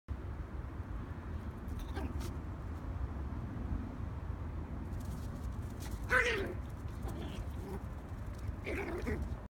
Yorkshire terrier attack ;)
Hawk sneaks up on Dublin